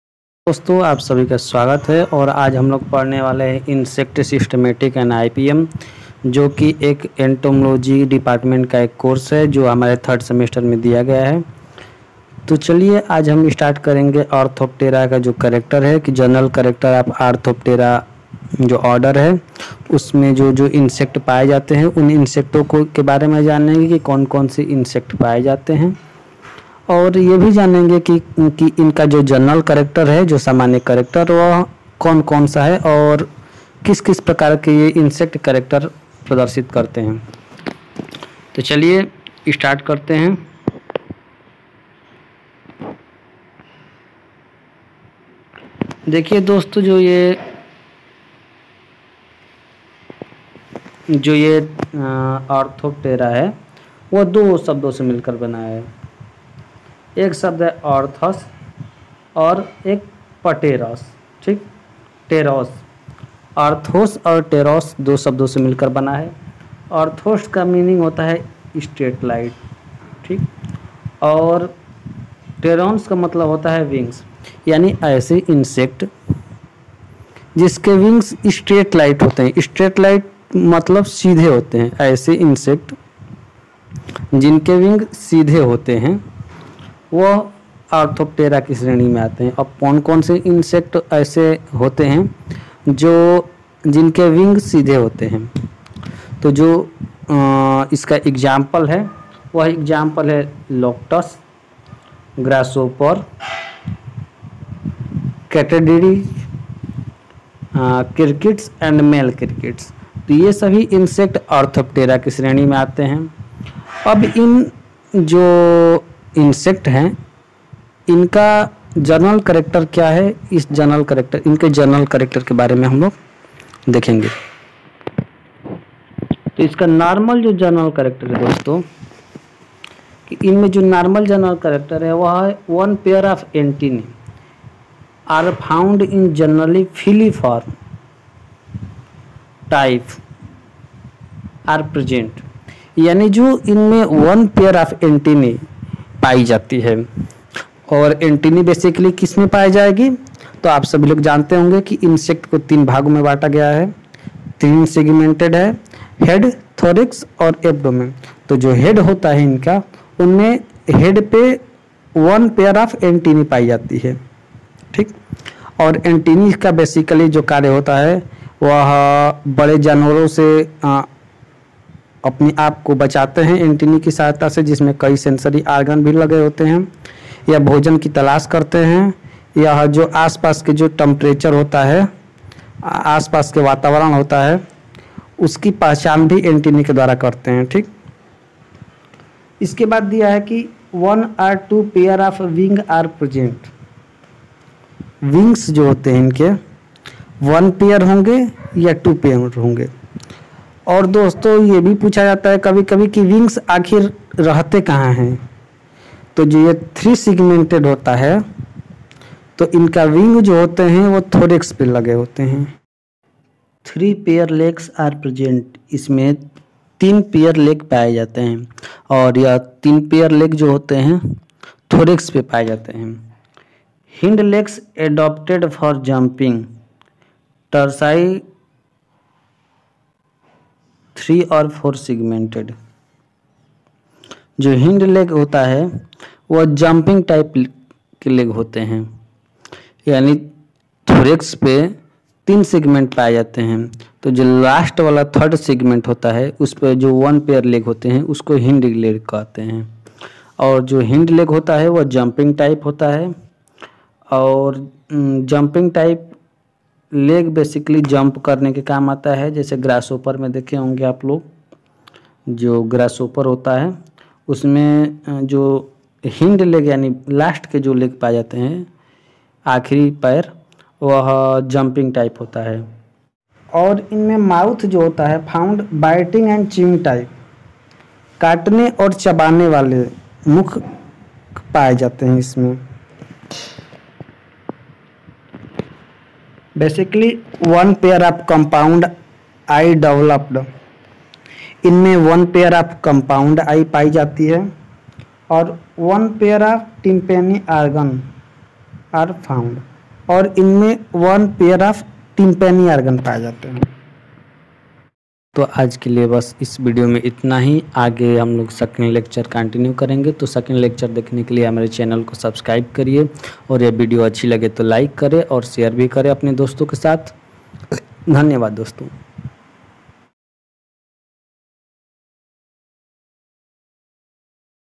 Order Orthoptera general cherecter | IPM | Grasshopper general cherecter#bhu
Order Orthoptera general cherecter

"Explore the Fascinating World of Orthoptera - Grasshoppers, Crickets, and Katydids!

In this video, we're shedding light on the Orthoptera order, which includes some of the most intriguing creatures in the insect world.

🦗 What is Orthoptera?
Orthoptera is an insect order that comprises a diverse group of species, including grasshoppers, crickets, and katydids. These creatures are known for their distinctive features and behaviors that set them apart from other insects.

🌿 Ecological Importance:
Learn about the crucial role Orthoptera plays in ecosystems. From being essential pollinators to providing a food source for various predators, these insects have a significant impact on the balance of nature.

🔬 Fascinating Facts:
Discover intriguing facts about Orthoptera's anatomy, behavior, and life cycle. Did you know that some species of crickets chirp faster in warmer temperatures? Find out more captivating insights!

🌍 Habitat and Adaptations:
Explore the habitats where Orthoptera thrives and how they've adapted to different environments. Whether it's the grasslands, forests, or even urban areas, these insects have found unique ways to survive and thrive.

🔍 Recent Research:
Stay up-to-date with the latest scientific discoveries related to Orthoptera. Researchers continue to uncover new information about these insects, expanding our understanding of their role in the natural world.

Join us as we embark on a journey into the world of Orthoptera and uncover the wonders these insects bring to our planet.

Feel free to modify and personalize the description as needed for your YouTube channel!